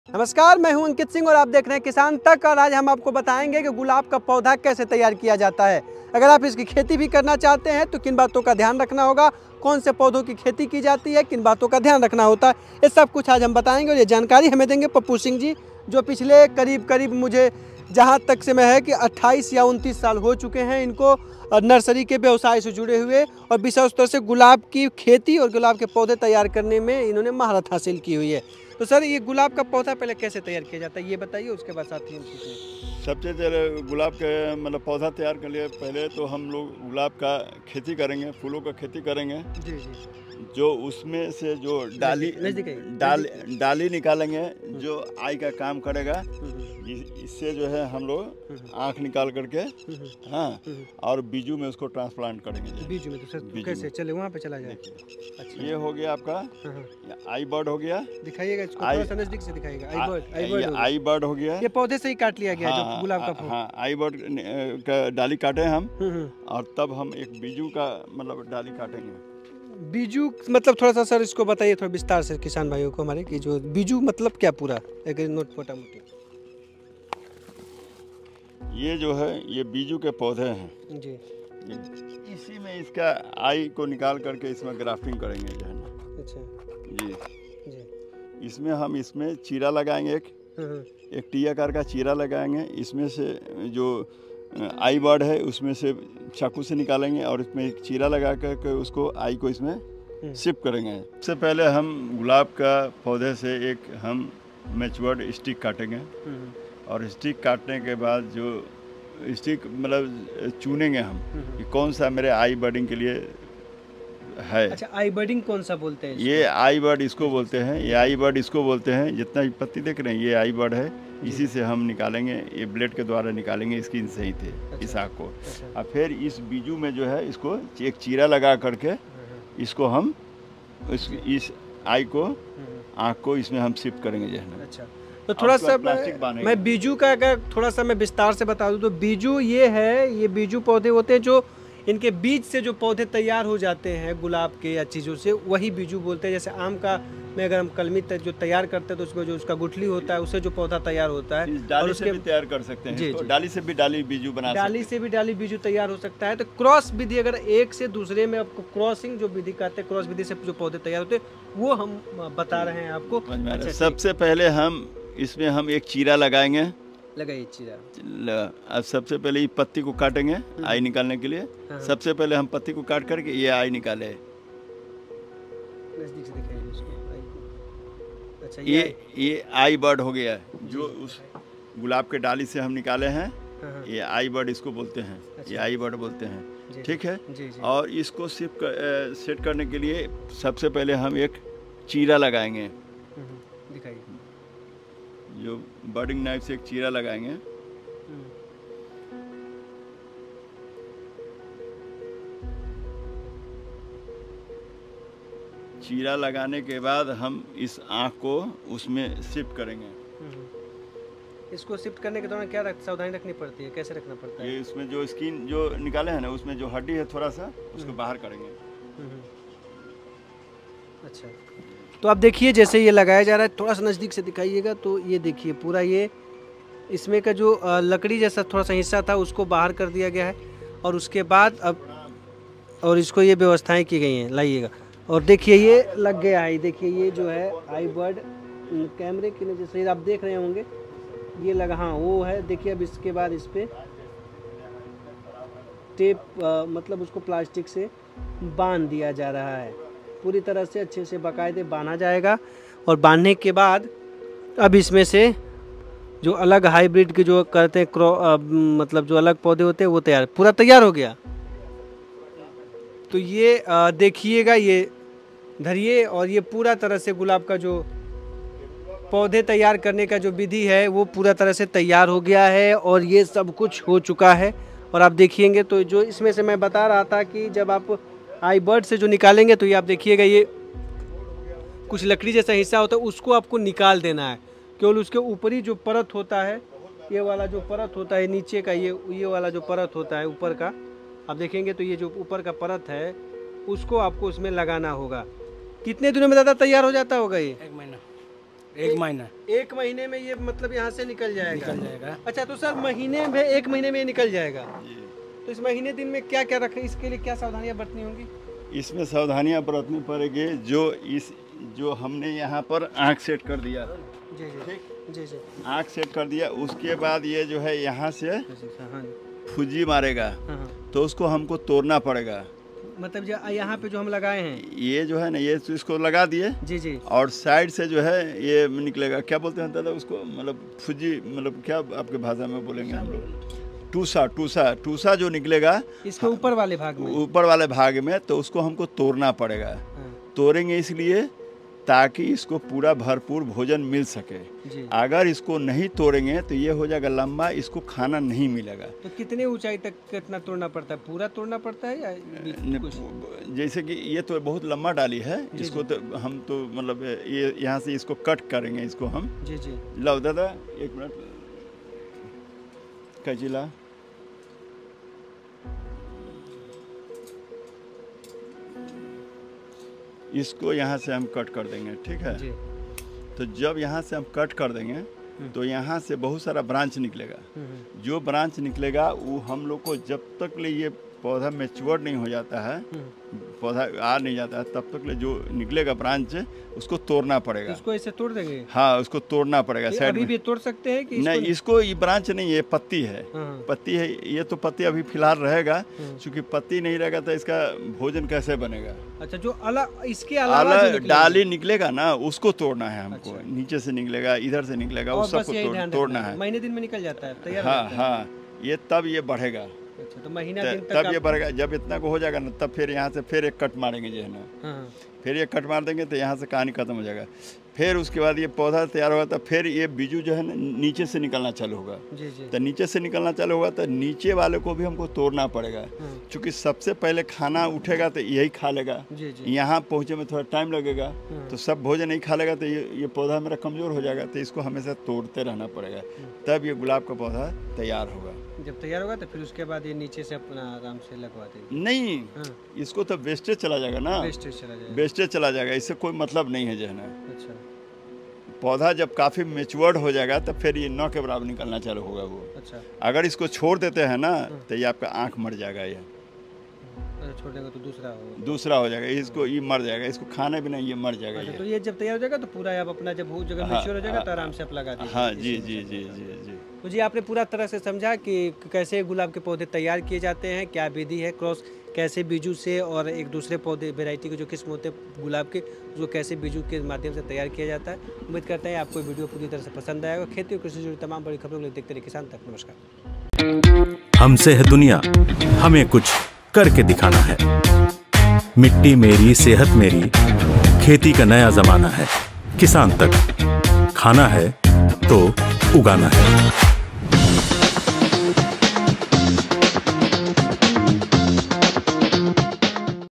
Gulab Ki Kheti: ऐसे तैयार करें गुलाब का पौधा, खूब आएंगे फूल | Kisan Tak
Rose Farming: गुलाब का पौधा छोटी सी बालकनी और आंगन से लेकर बड़े-बड़े बागानों की खूबसूरती बढ़ता है. साथ ही इसकी खेती (Gulab Ki Kheti) से भी किसान बढ़िया मुनाफा कमा रहे हैं. सिर्फ किसान ही नहीं नर्सरी का बिजनेस  (Nursery Business) कर रहे लोगों को भी गुलाब का पौधा अच्छी कमाई करवा रहा है. पटना में ऐसी ही एक नर्सरी के मालिक पप्पू कुमार से किसान तक की टीम ने मुलाकात की. इस दौरान उन्होंने बताया कि गुलाब की खेती के लिए ऊंची जमीन चाहिए होती है. बीजू के पौधे में हाइब्रिड गुलाब तैयार किया जाता है. बीज से तैयार किए पौधों को बीजू कहते हैं. सबसे पहले गुलाब की डाली से आई बर्ड निकालते हैं. आगे का प्रोसेस जानने के लिए देखें ये वीडियो

Credits:
Reporter
Producer
Editor